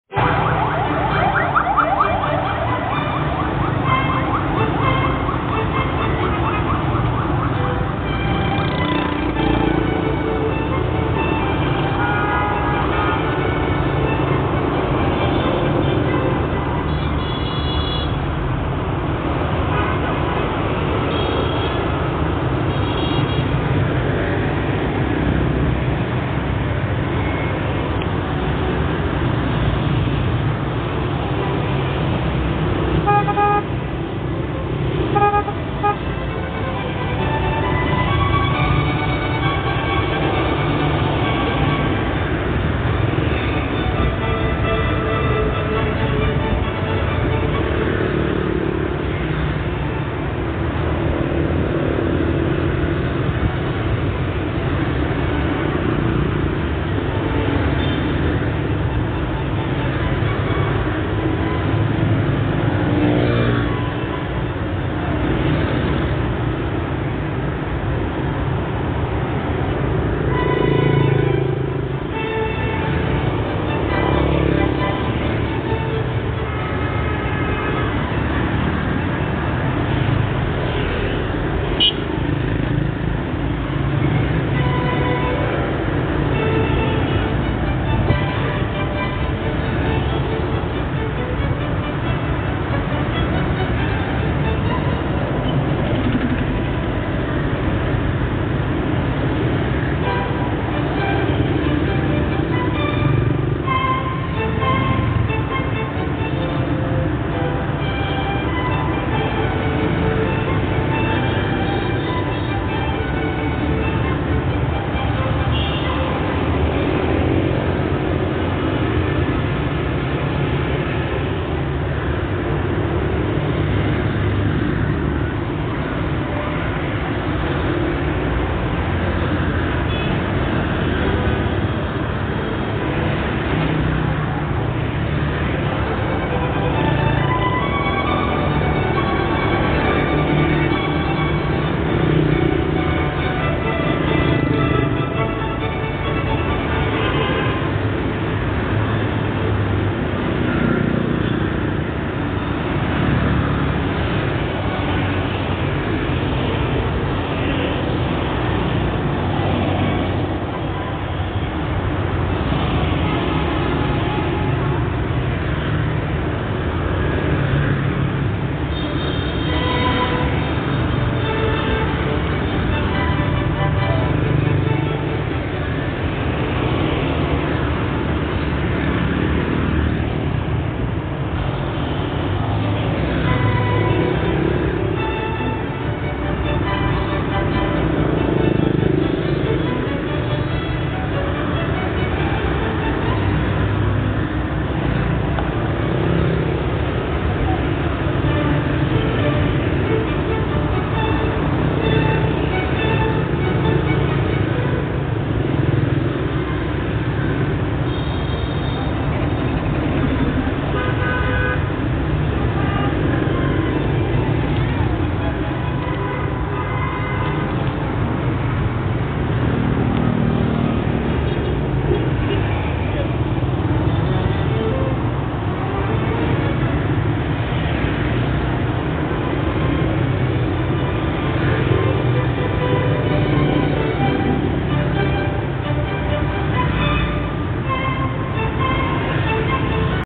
Trời Lạnh, Đầu Nóng, Đường Đông
vivuhanoipho Bác tài căng thẳng với nhau